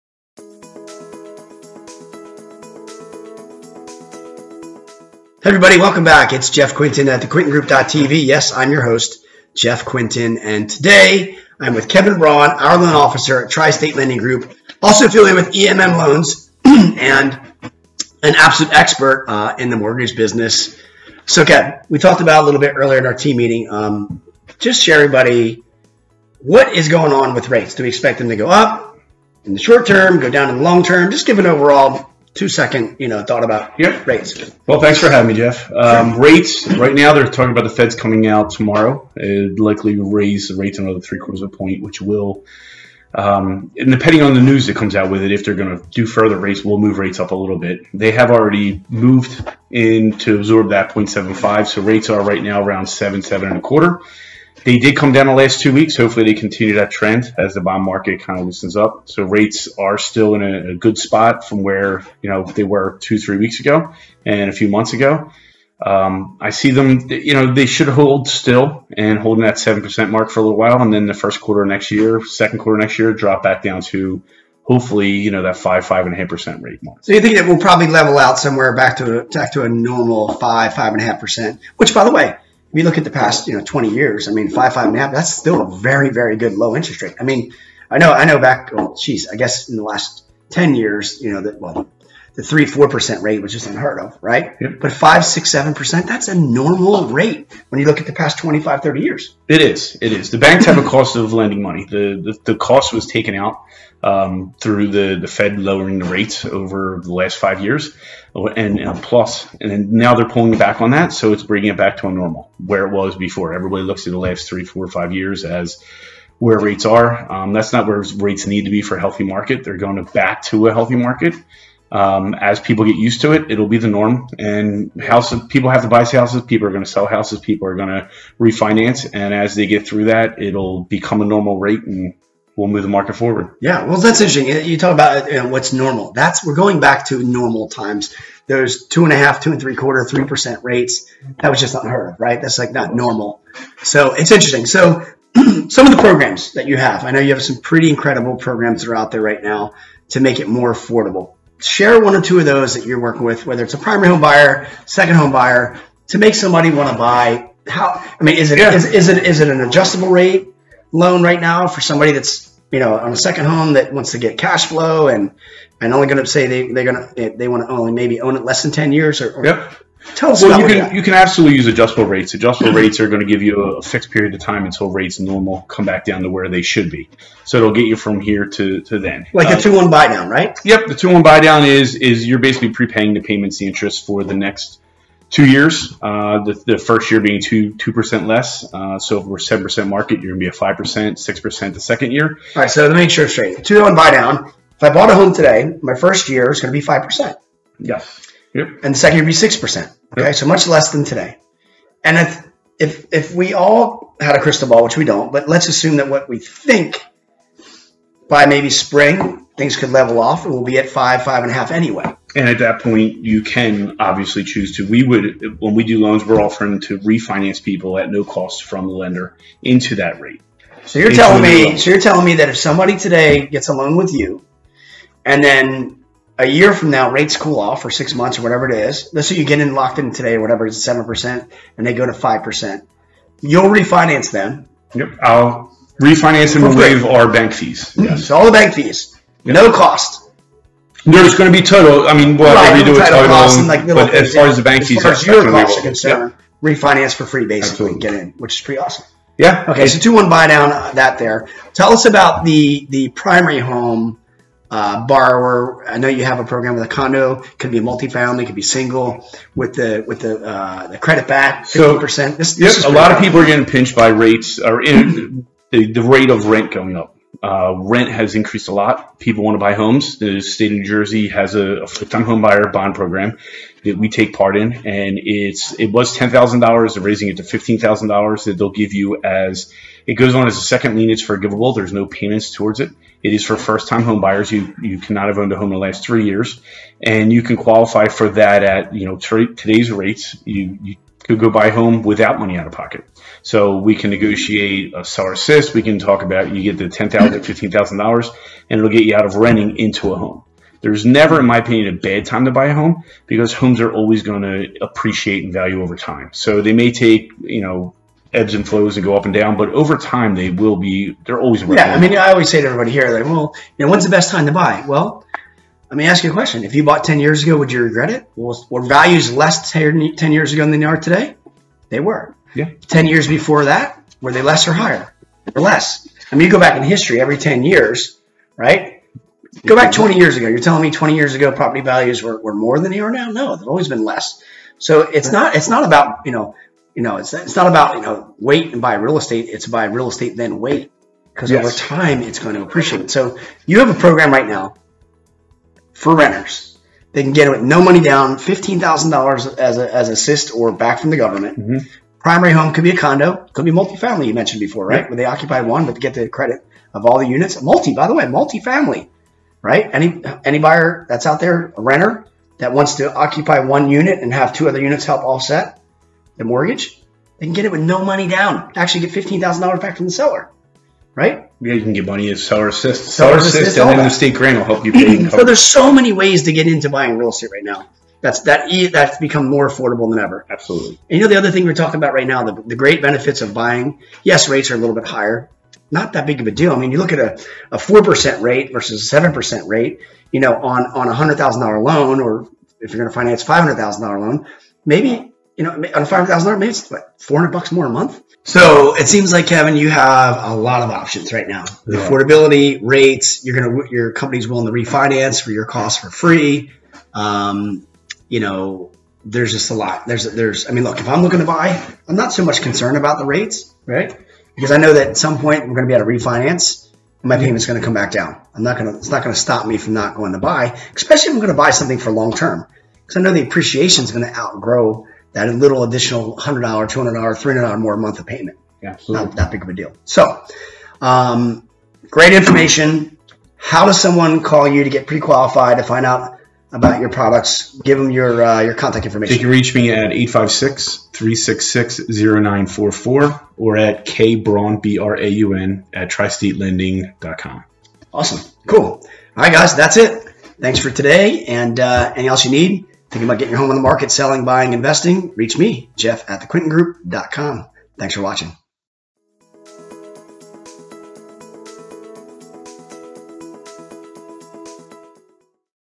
Programs That Will Help Homebuyers
There are a lot of buyers out there today that are very skeptical—are you one of them? If so, that is completely understandable, news about interest rates can be very alarming. However, you should know that you have options, as this market is full of opportunities for buyers. To tell us about them and bring clarity to the interest rate confusion is expert Kevin Braun from the TriState Lending Group of EMM Loans.